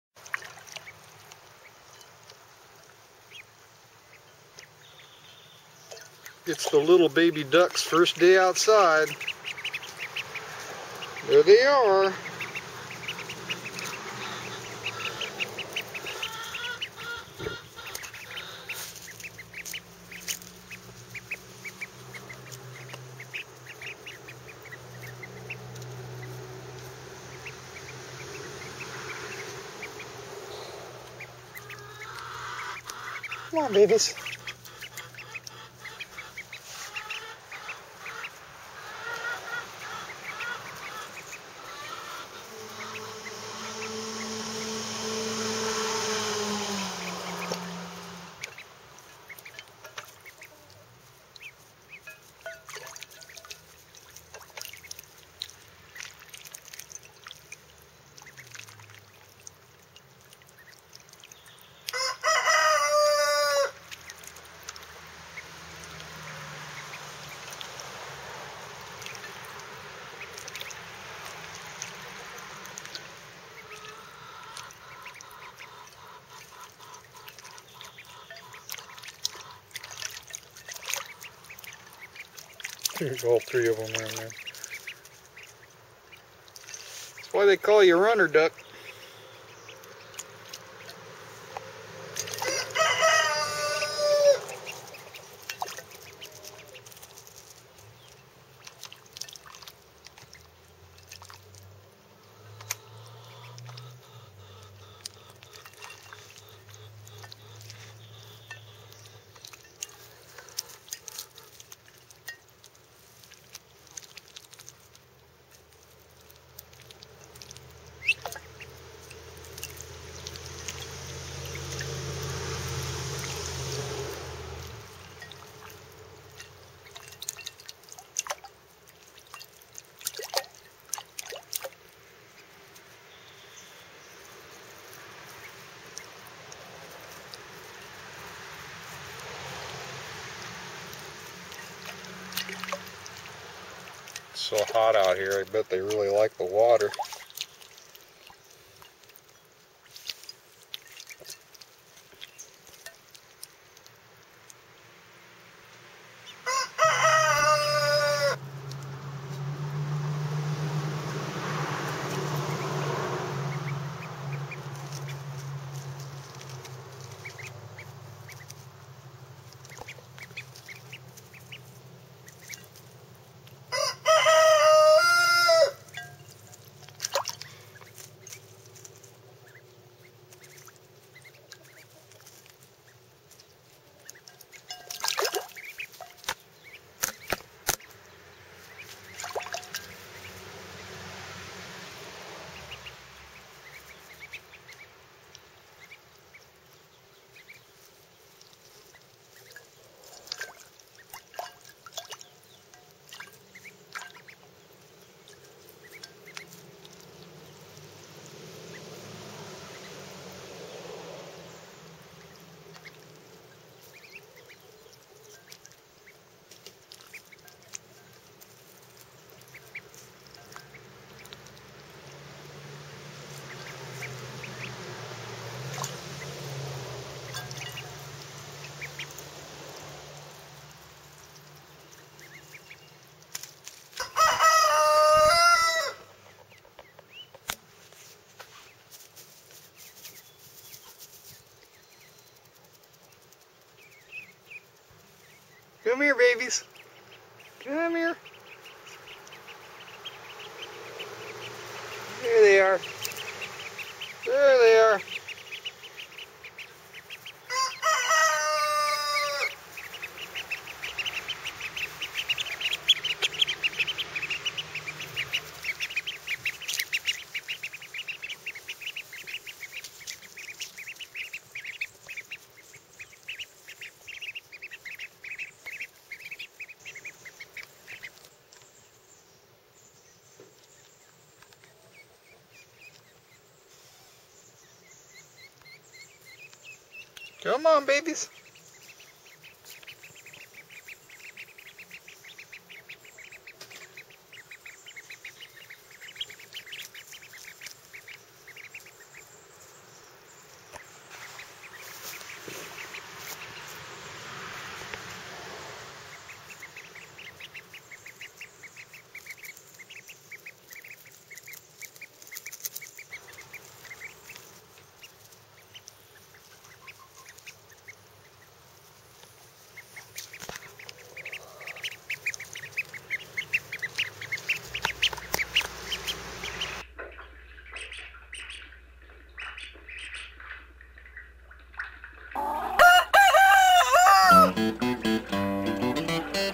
My purebred baby Runner Ducks enjoying their first day outside! =D
My purebred runner ducks enjoying a dip on a hot day for their first time outdoors! =) Bullwinkle, Phyllis, and Mollie are about two and a half weeks old showing their cuteness in a way that only a happy and loved duck could do! They are hand tame and very demanding on their mother...ME!! Lol! :)